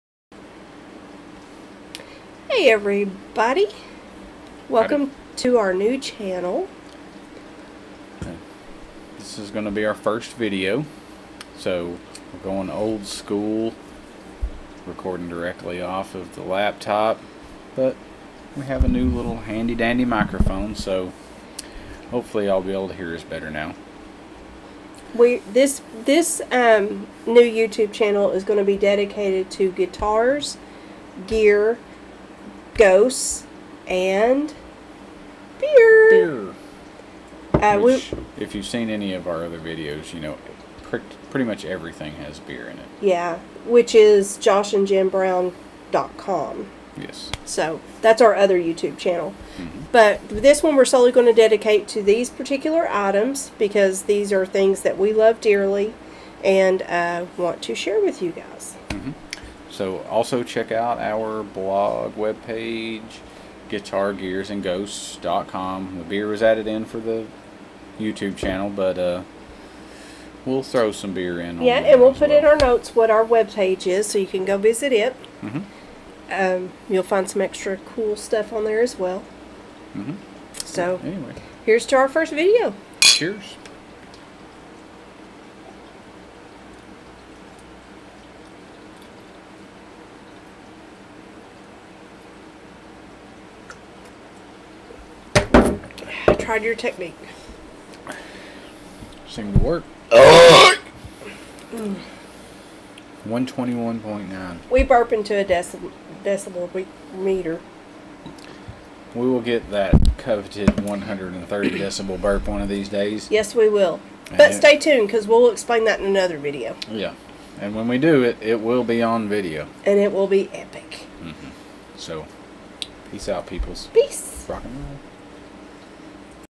First Chug on the New Channel
Our first video on our new YouTube channel. A quick little note about the new channel, followed by a chug and, of course, a sweet burp.